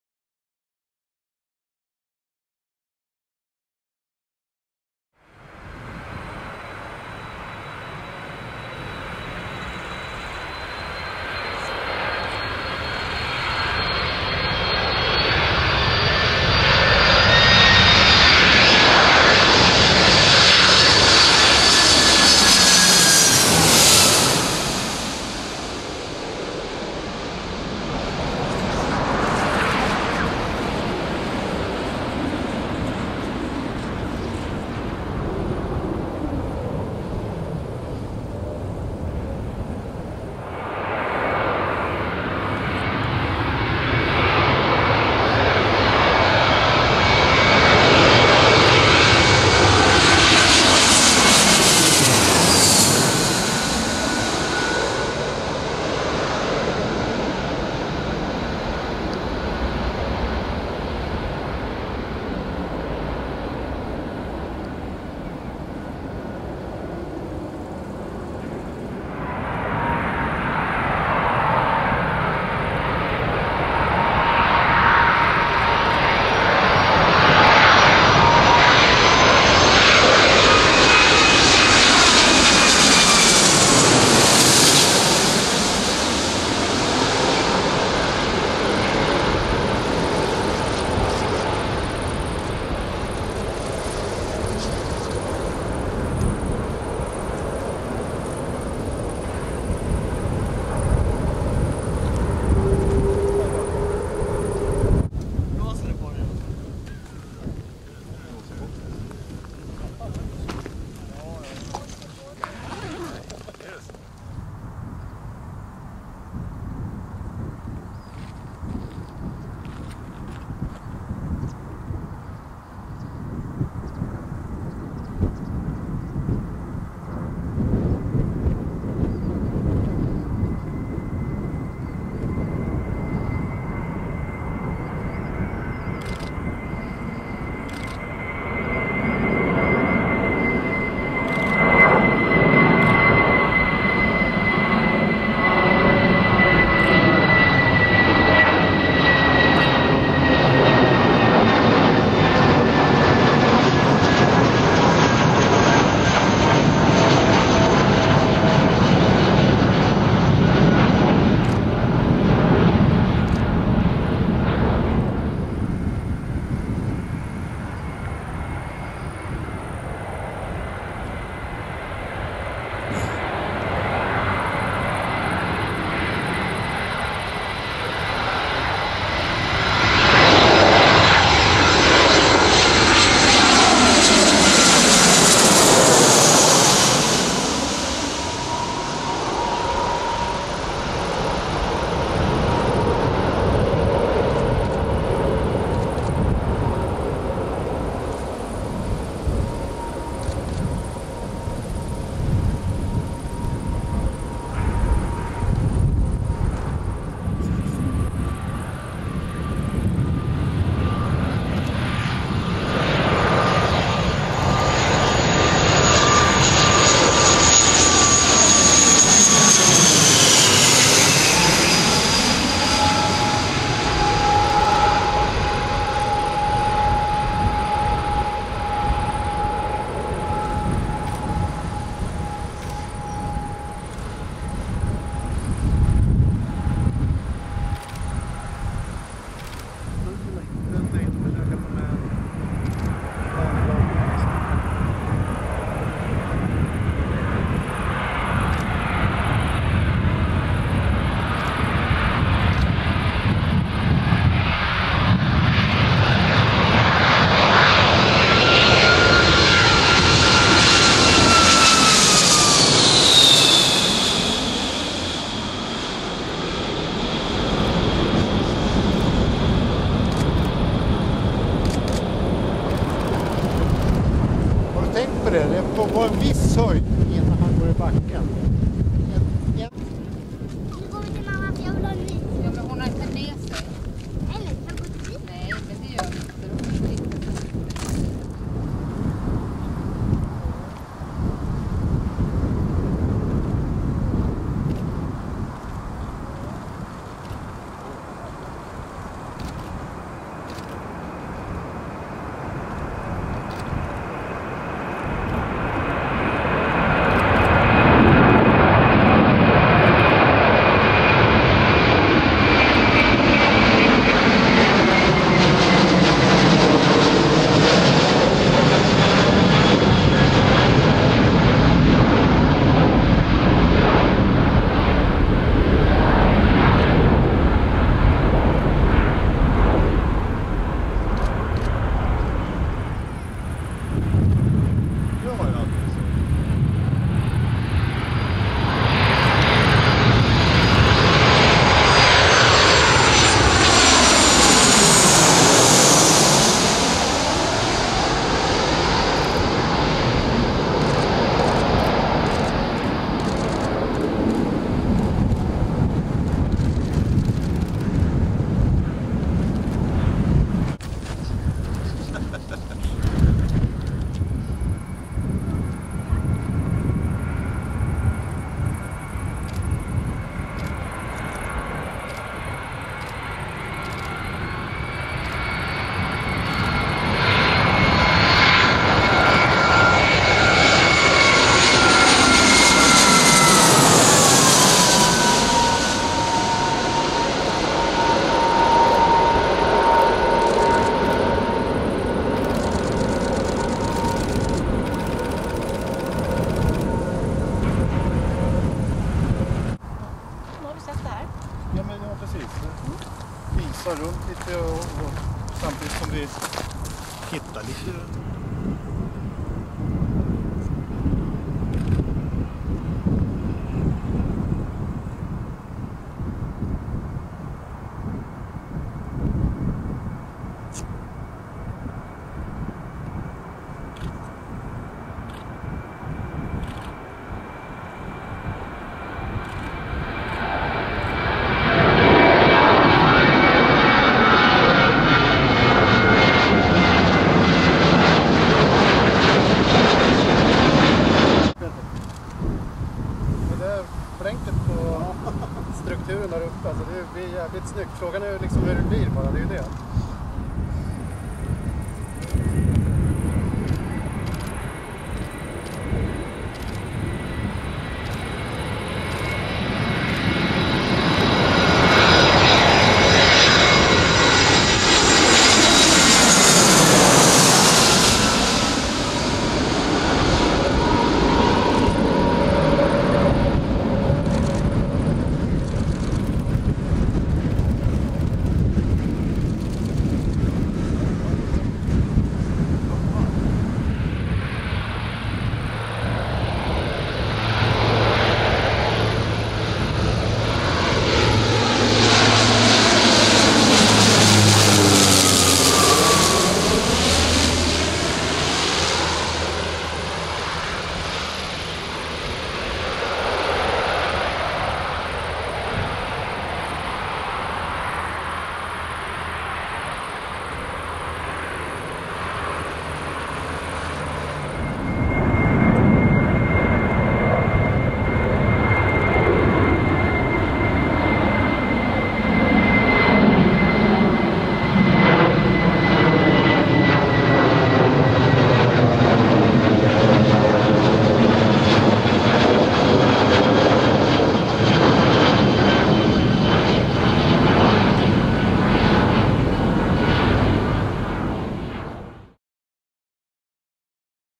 Stockholm Arlanda, august 5, 2012
Spotter-meeting at Stockholm Arlanda (ESSA) august 5, 2012.
A nice day for us photographers, but a little crosswind for the pilots.